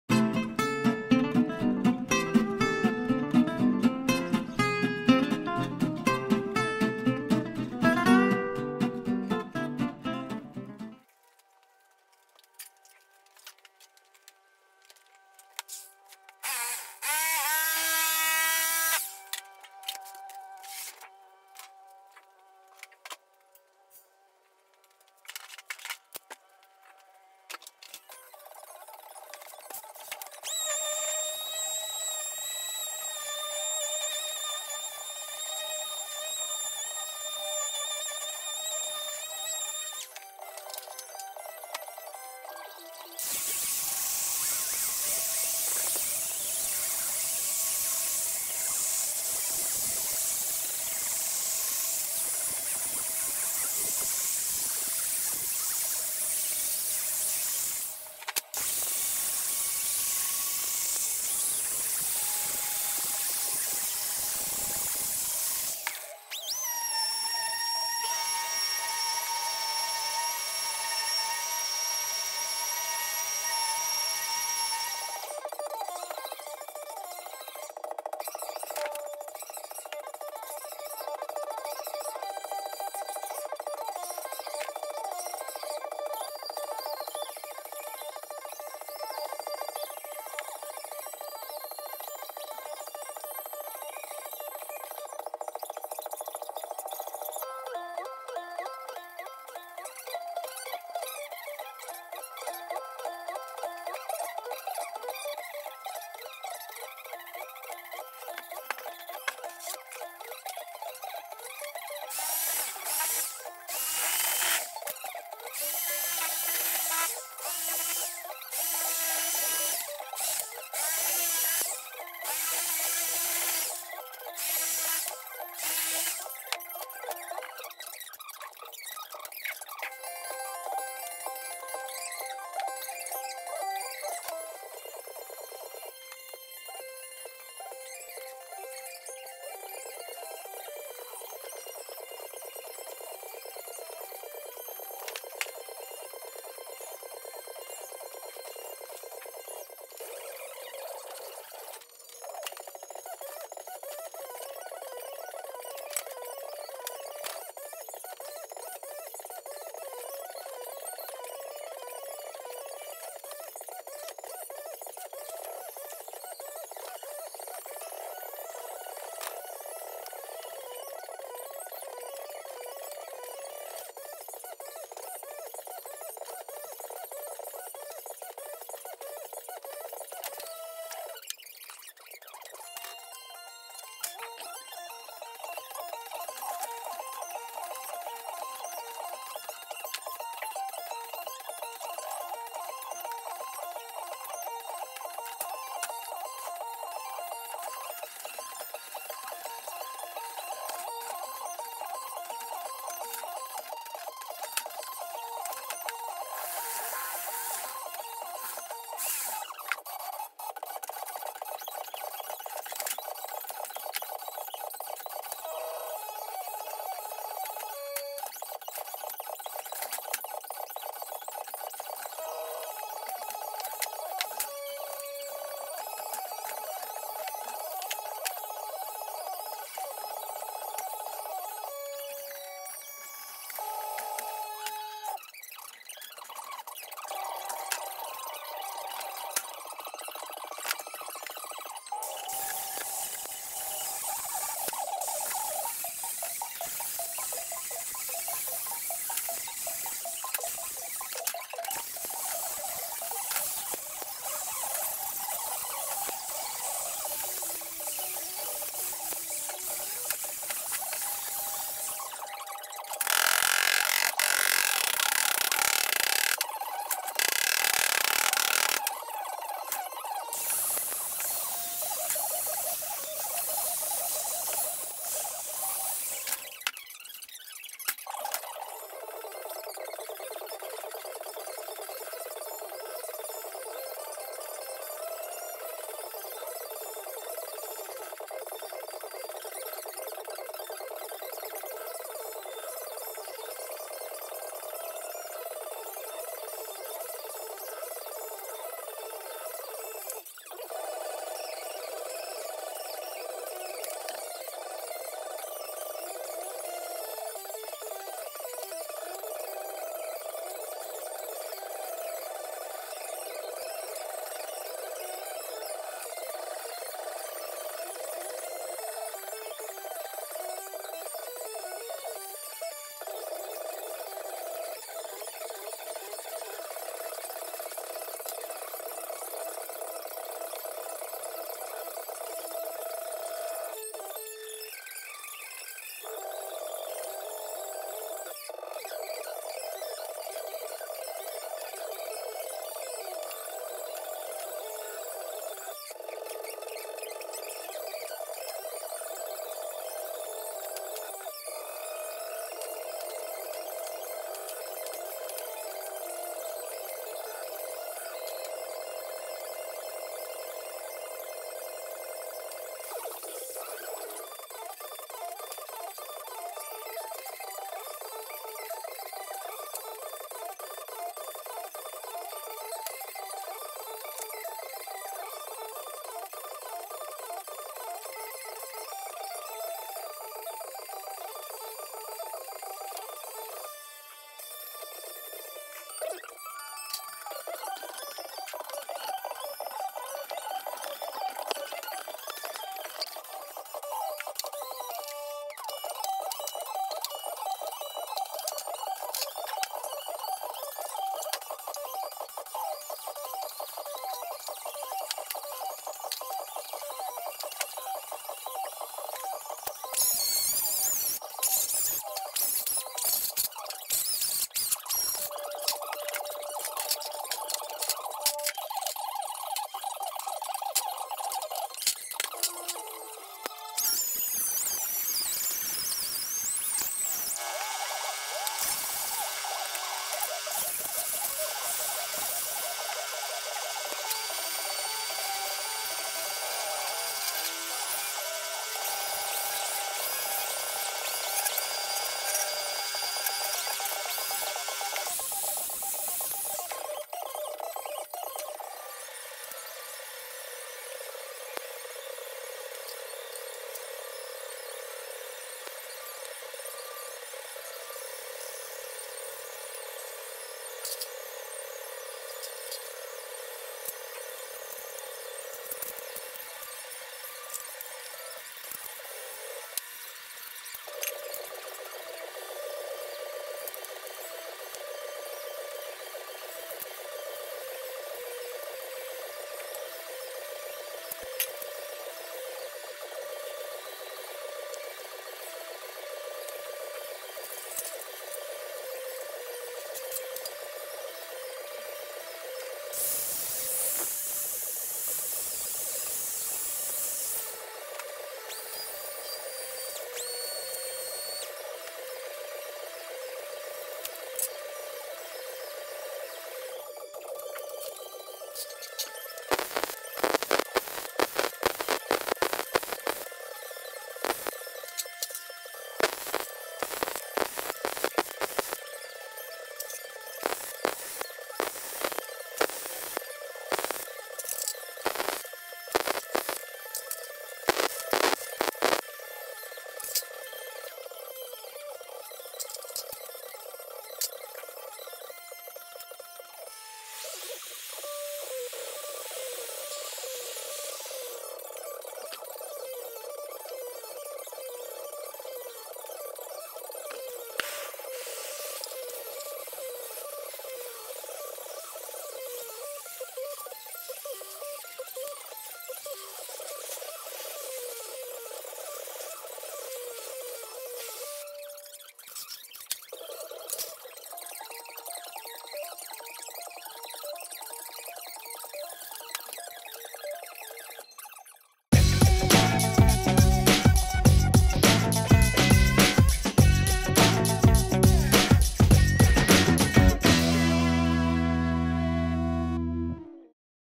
Making A Fish
This video is about making a sheetmetal fish to hang on the wall.